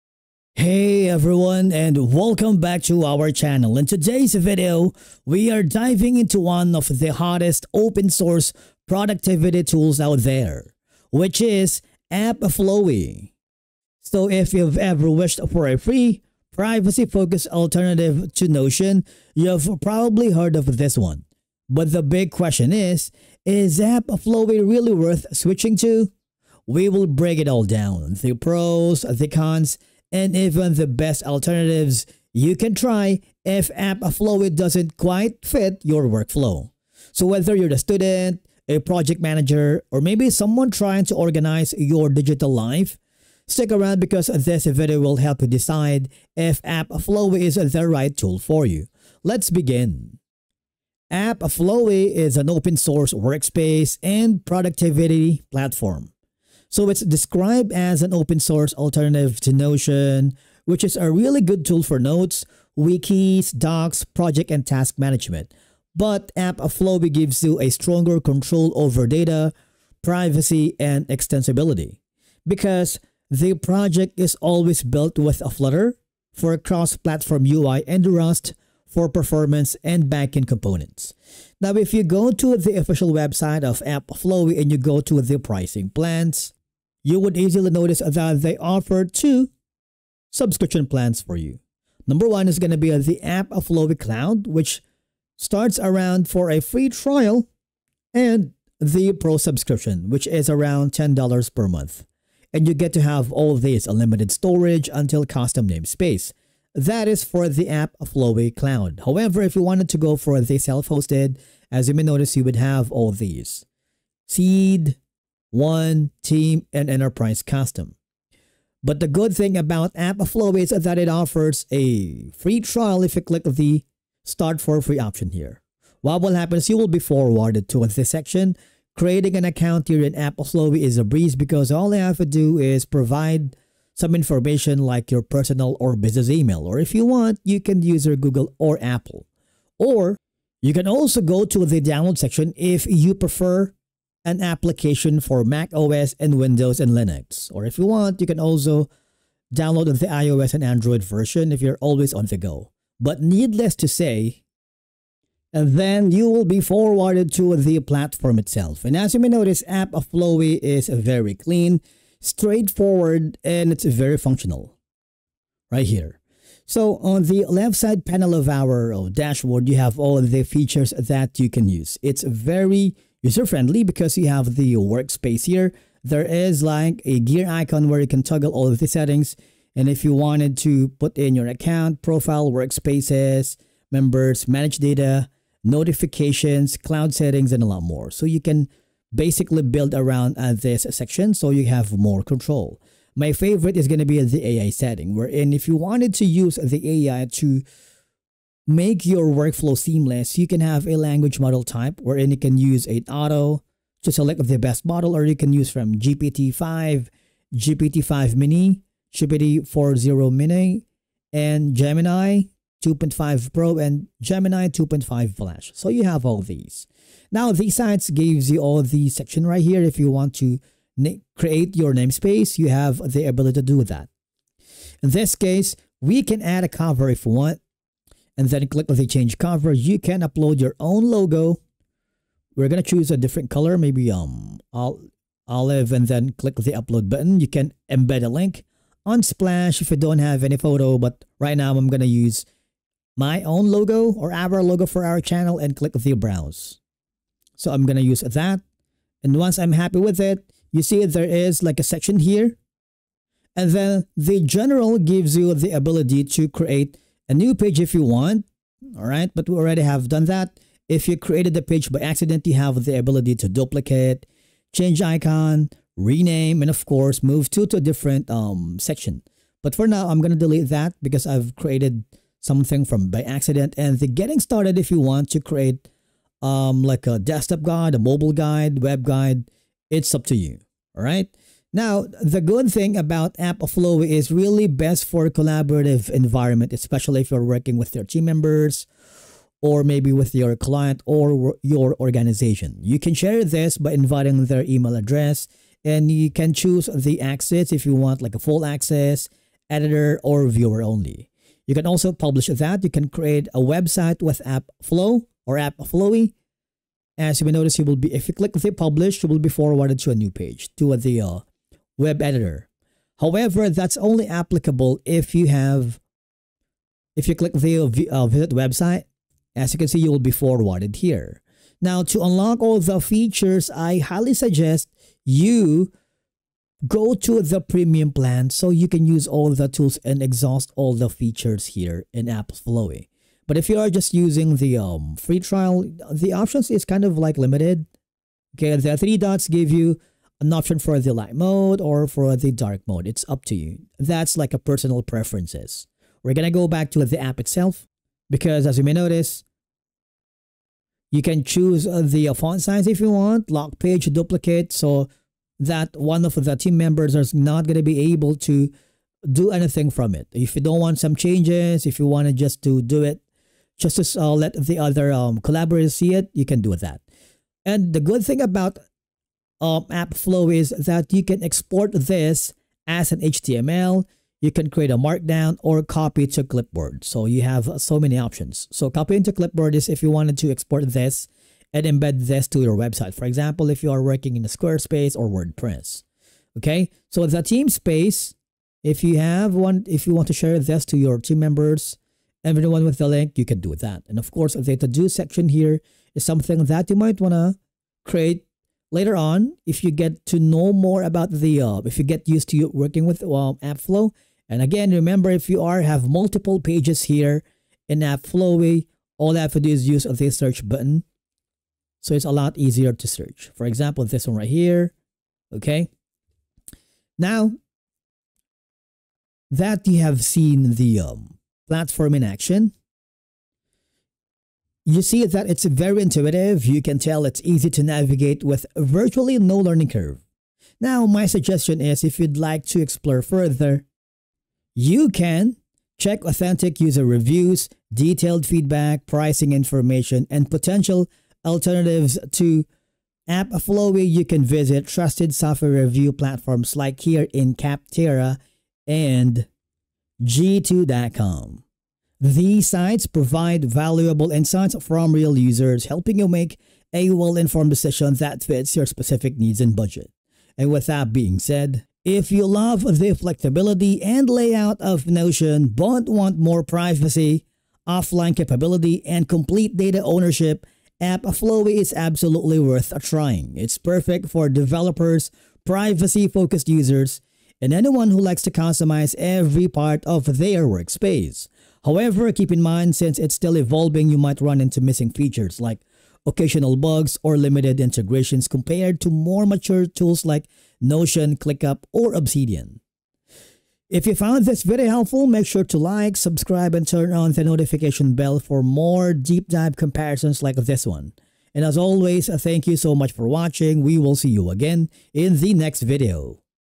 AppFlowy Review & Demo | Pros, Cons, Features & Alternatives
In this beginner tutorial and training video, we show you how to install and use AppFlowy software. We show you how to create an account and get started, how to use all of the core features in this demo, and give quick review of the software.